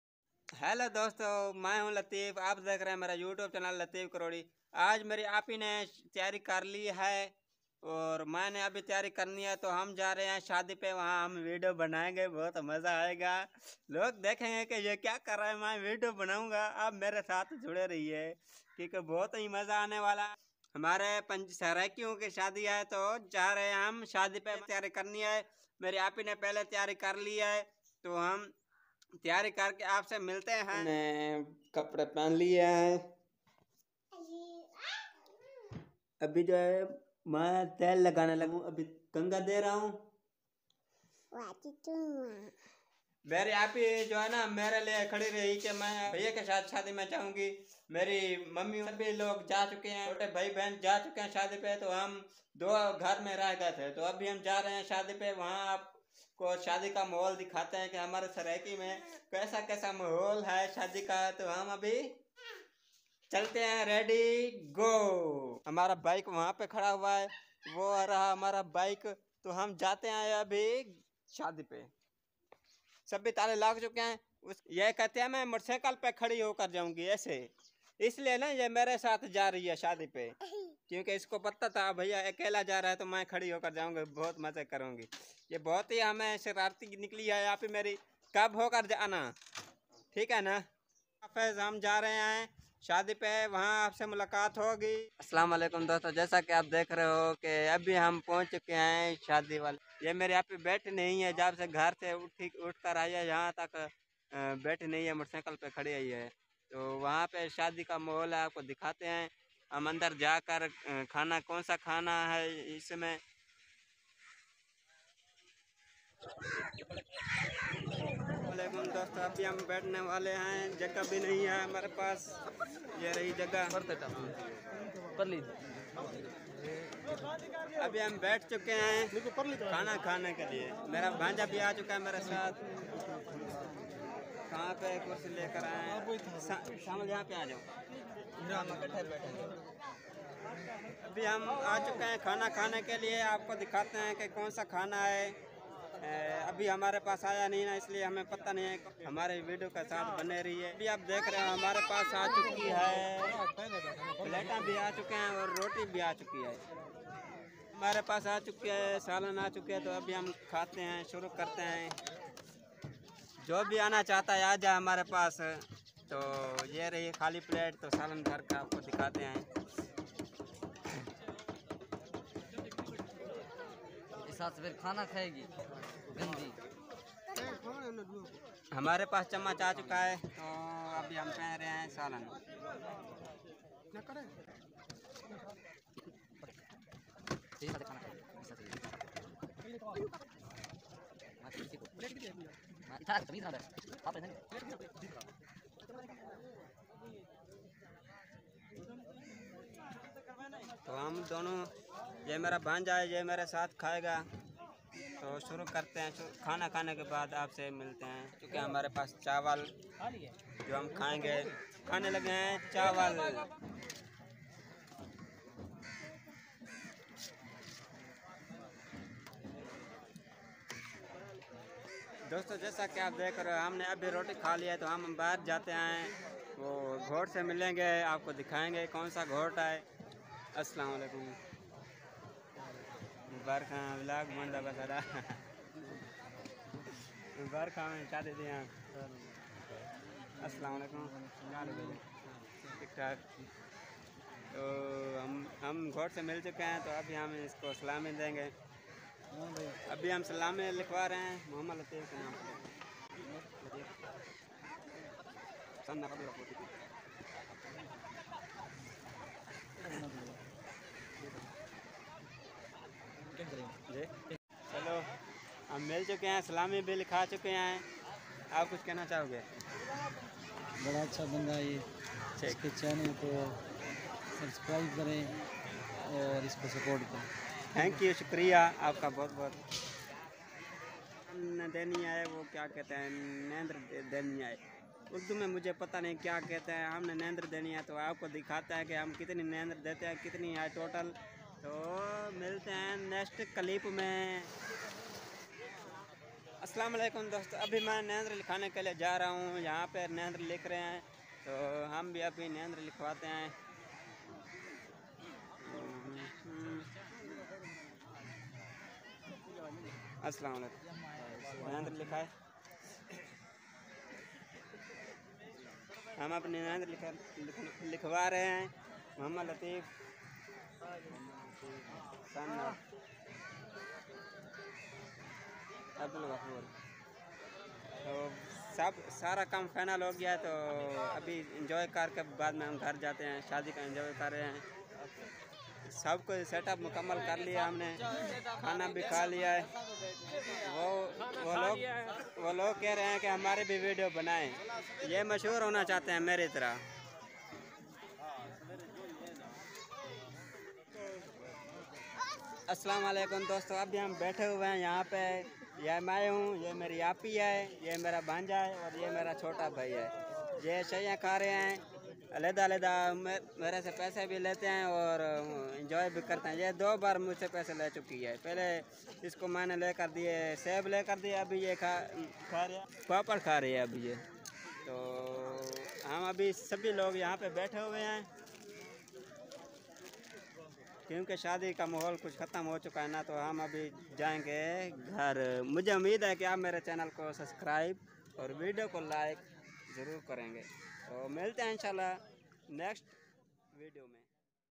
gaon ki Shaadi|| dehat ki shaadi|| latifkarori|| intertanment
yahaan pa app subko Shaadi ka mahool dikayen gay|| latifkarori|| ہمارے گاؤں کی شادی